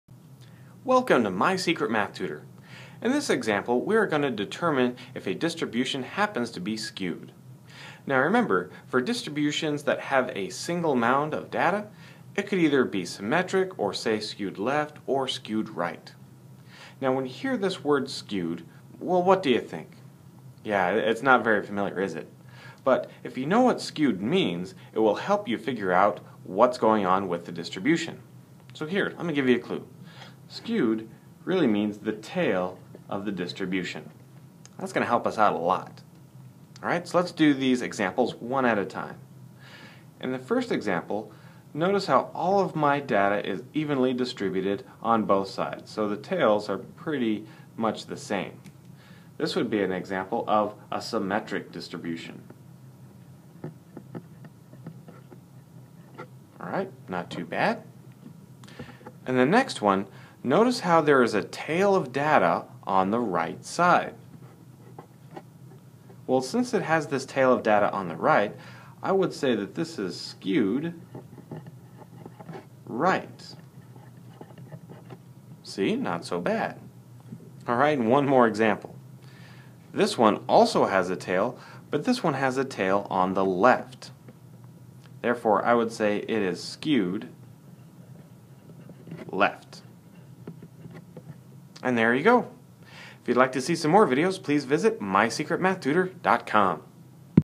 Statistics - Reading the shape of a distribution
In this example we look at reading the shape of a distribution.  More specifically we look at if it is skewed left, right, or is symmetric.  Remember that the skew is the tail of a distribution.